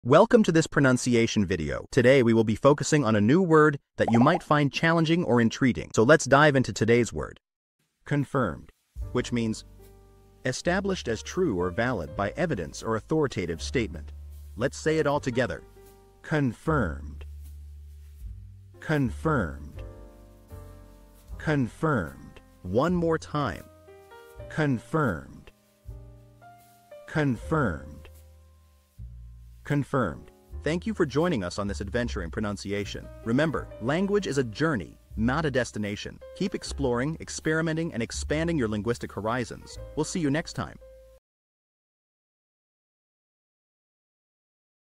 How to pronounce Confirmed
Master the Pronunciation of 'Confirmed' - which means : Established as true or valid by evidence or authoritative statement. 📝 with @PPrononciation 🎓🗣️ - [Your Guide to English]

Learn to pronounce 'Confirmed' perfectly in English with our easy and engaging tutorial by @PPrononciation! 📖📽️ In this comprehensive video, we provide you with native speaker examples 🗣️👂 and phonetic breakdowns 🎵 to ensure you get the pronunciation just right.

How to say Confirmed in English